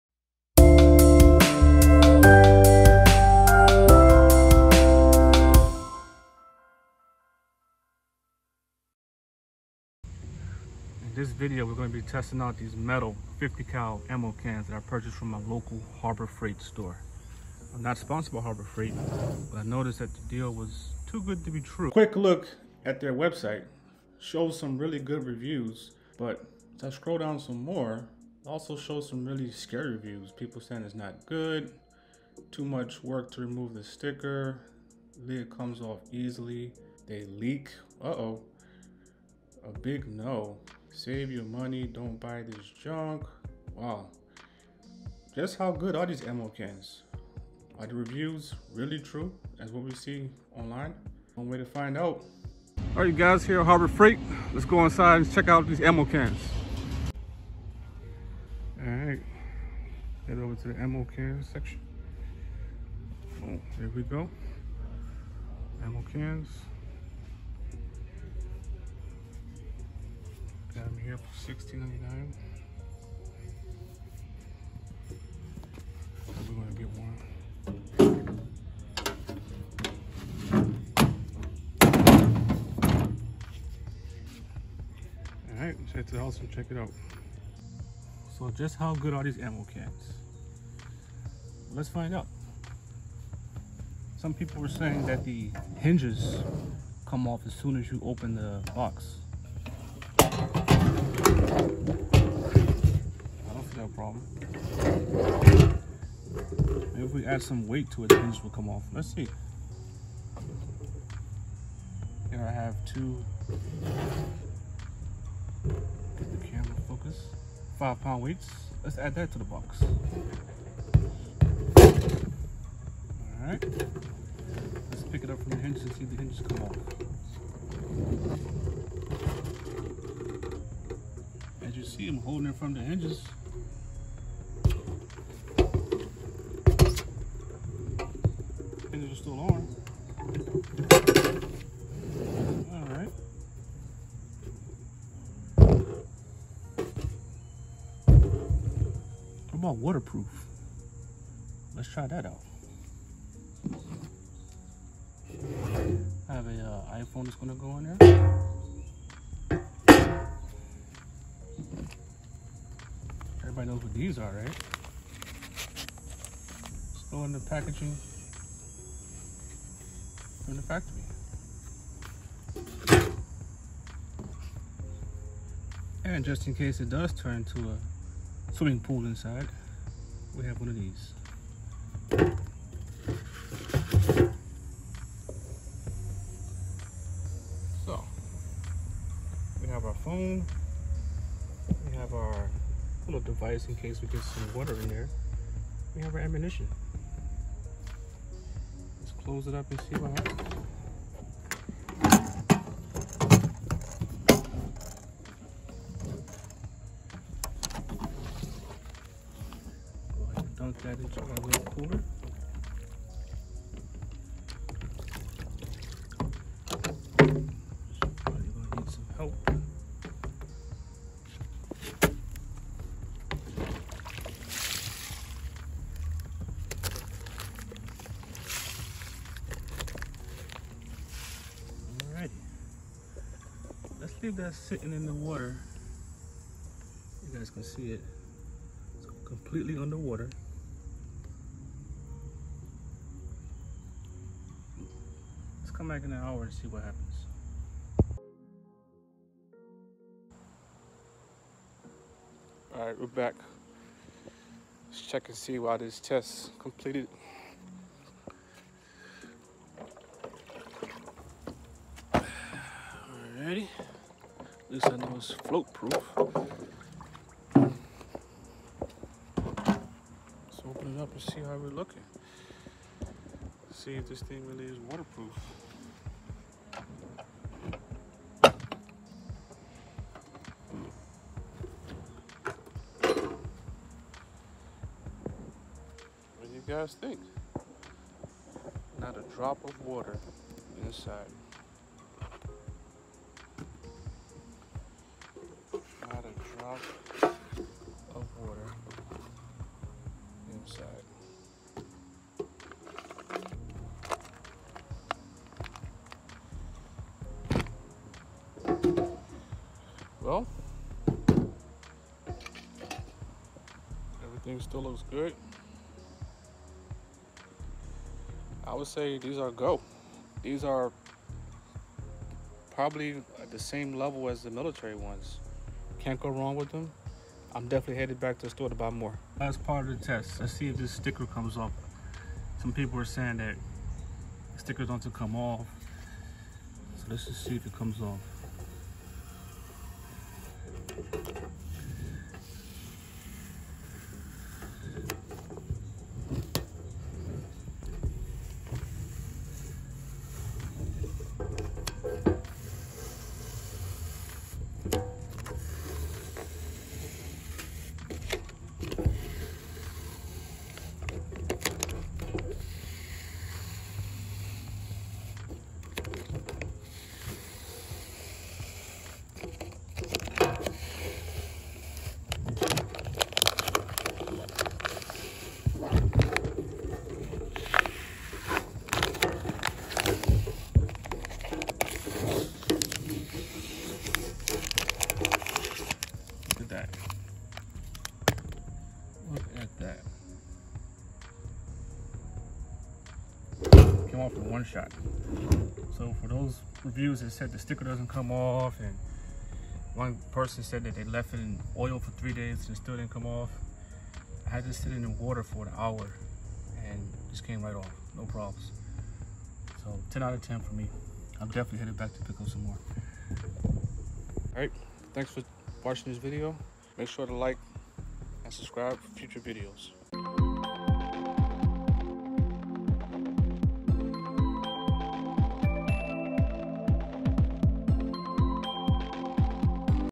Harbor Freight Ammo Cans - How Good Are They?
I purchased ammo cans from Harbor freight and put them to the test. Are these as good the military surplus cans?